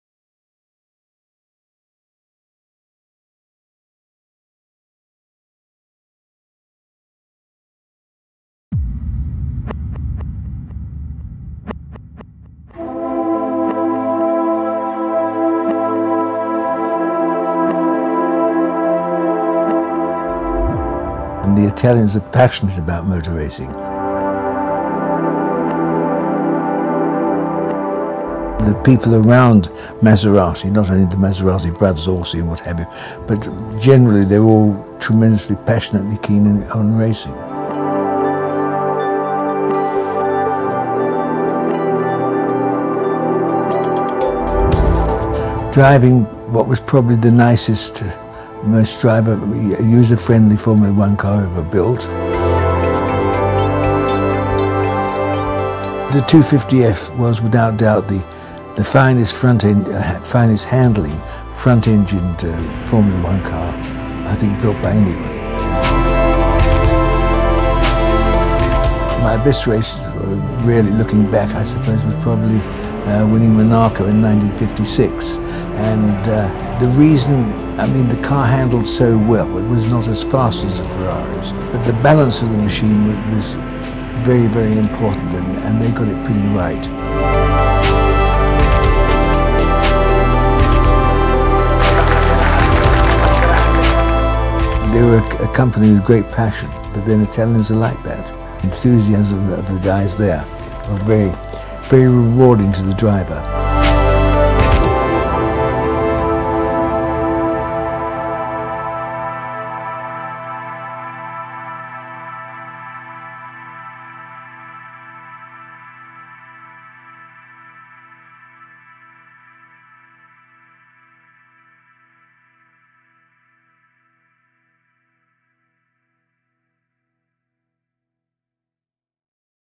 Sir Stirling Moss on Maserati
Sir Stirling Moss, one of the greatest driver of all the time, reminisces his driving career with his favourite car: Maserati 250F.